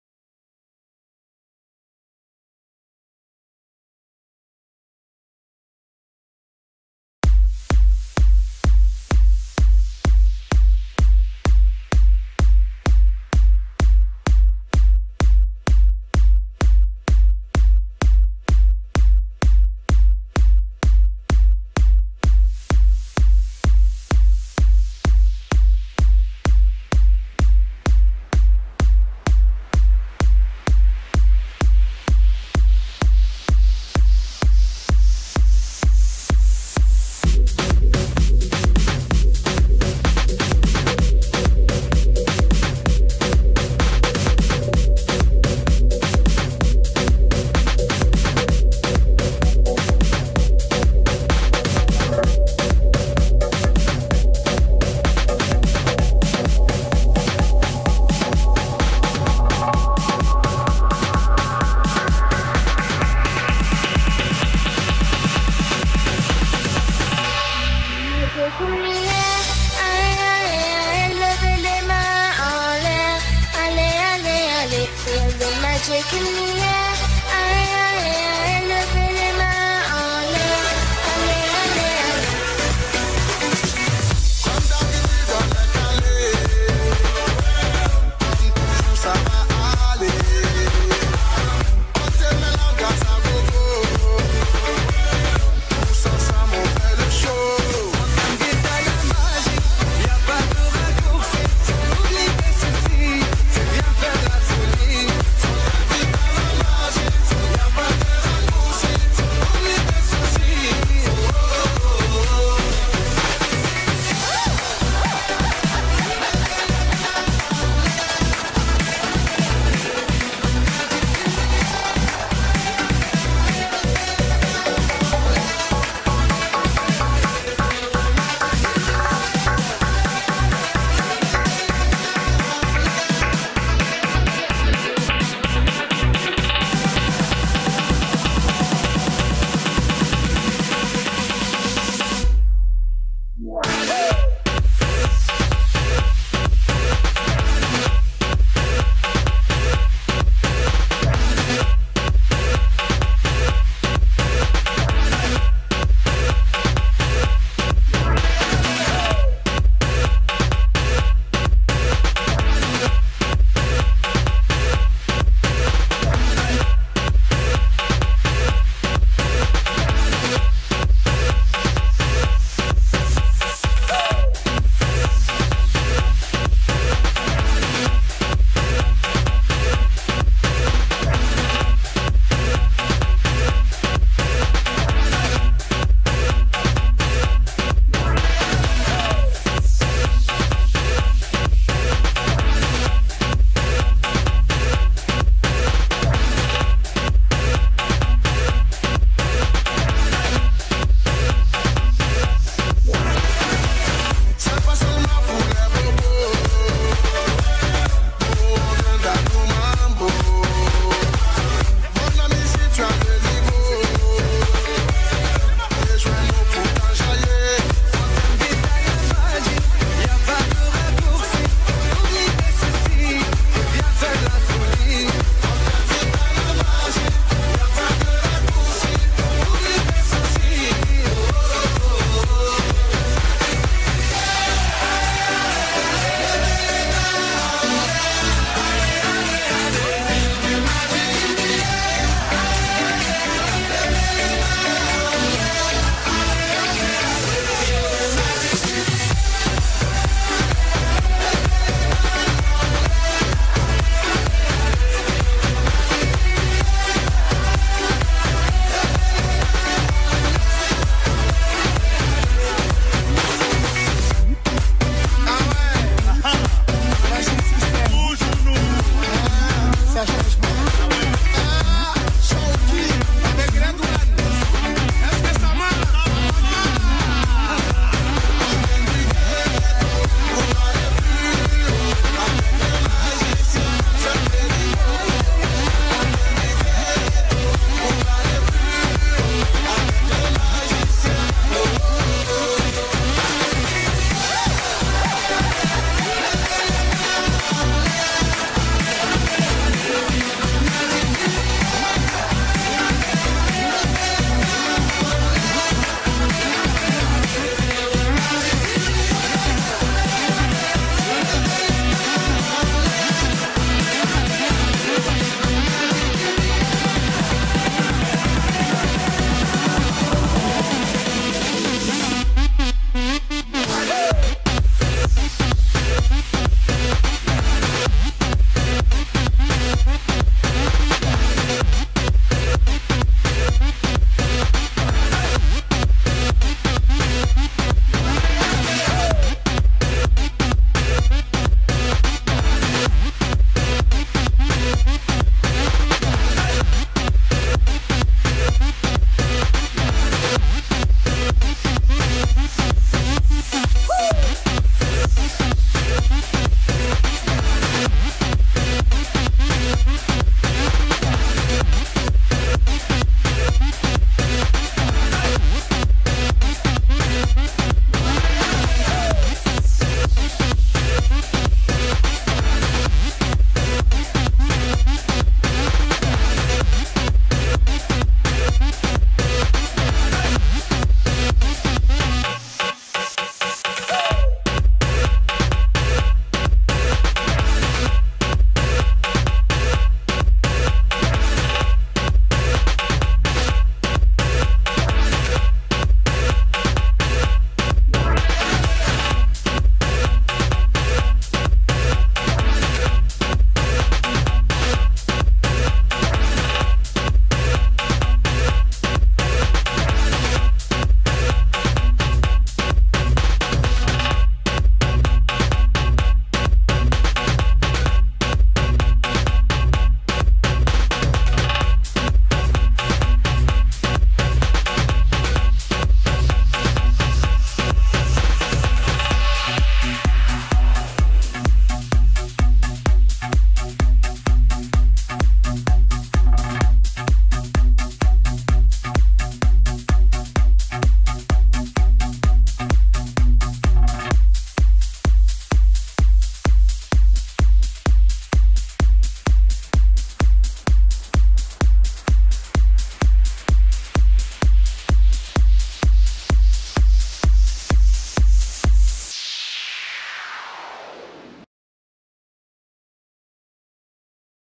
Les enfants font leur art martial II. Parcours de motricité. Exercices recréatifs.
Dans cette suite, les enfants ont été invités à faire preuve d'encore plus d'imagination, de créativité et de libre arbitre. L'objectif de la séance était une fois de plus de favoriser le développement d'habilités motrices diverses en restant dans un esprit bon enfant.